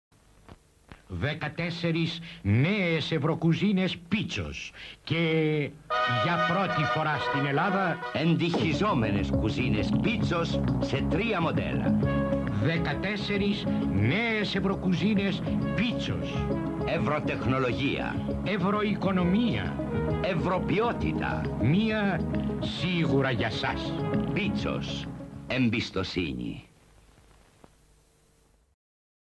ΔΙΑΦΗΜΙΣΗ " ΠΙΤΣΟΣ " ( 1981 )
Ο ηθοποιος Γιωργος Βρασιβανοπουλος , γνωστος και ως "μπαμιας" απο την "Χαρτοπαικτρα", διαφημιζει τις νεες εντοιχιζομενες "ευρωκουζινες"  της ελληνικης εταιρειας ηλεκτρικων ειδων "ΠΙΤΣΟΣ" .η Ελλαδα λιγους μηνες πριν αποτελουσε το 10ο μελος της ΕΟΚ και  οποιαδηποτε αναφορα στην Ευρωπη γινοταν αυτοματα "πιασαρικη" και της "μοδας" .